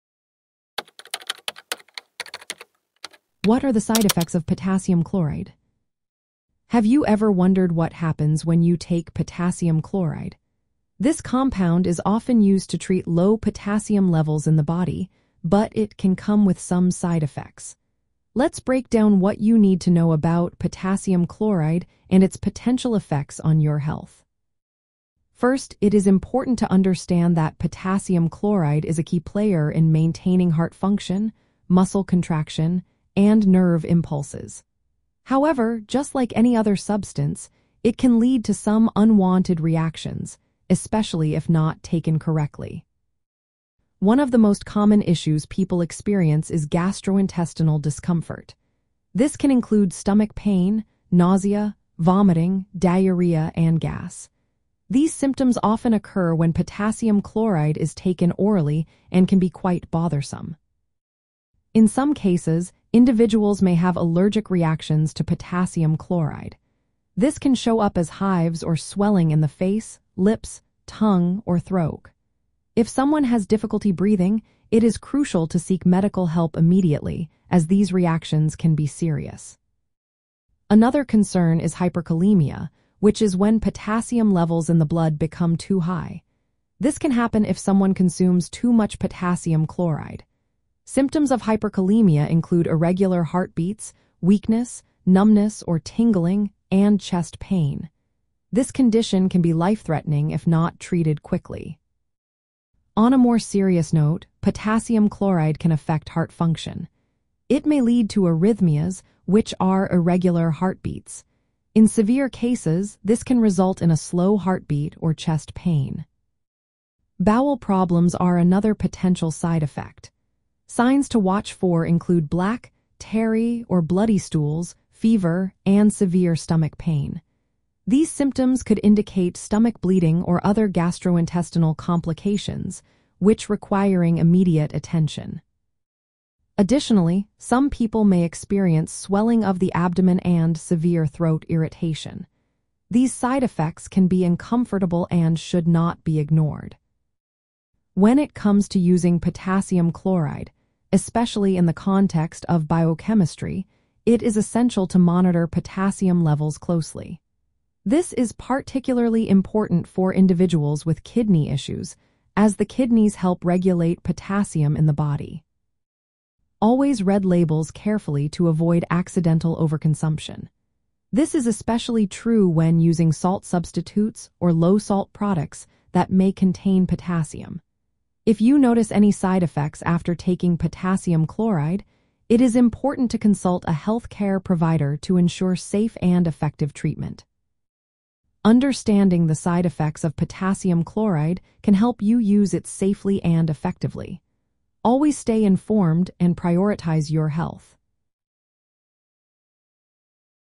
What Are The Side Effects Of Potassium Chloride? - Biology For Everyone
What Are The Side Effects Of Potassium Chloride? In this informative video, we will cover the important aspects of potassium chloride and its side effects. Potassium chloride is commonly utilized to address low potassium levels in the body, but it’s essential to be aware of the potential reactions that may occur. We will discuss the common gastrointestinal issues that can arise, such as stomach pain and nausea, as well as more serious allergic reactions that may require immediate medical attention.

Additionally, we will touch on the risks of hyperkalemia, a condition characterized by elevated potassium levels in the blood, and how it can impact heart function. Signs of hyperkalemia, such as irregular heartbeats and weakness, are critical to recognize. We will also highlight other side effects, including bowel problems and throat irritation, that can occur with potassium chloride use.

Monitoring potassium levels is vital, especially for individuals with kidney concerns. Understanding these side effects can help you use potassium chloride safely and effectively. Stay informed and prioritize your health by watching this video.